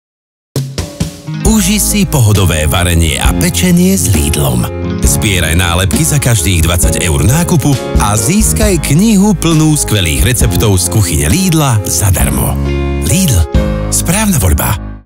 🎄📕 Získaj knihu plnú receptov z Kuchyne Lidla | Lidl Slovensko
Uži si pohodové varenia a pečenie s Lidlom. 🎄 Získaj kuchársku knihu plnú skvelých receptov z Kuchyne Lidla zadarmo. Za každých 20 € nákupu dostaneš v Lidli jednu nálepku a keď ich nazbieraš 8, knihu ti odovzdáme pri pokladnici bez doplatku. Akcia trvá od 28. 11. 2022 do vyčerpania zásob.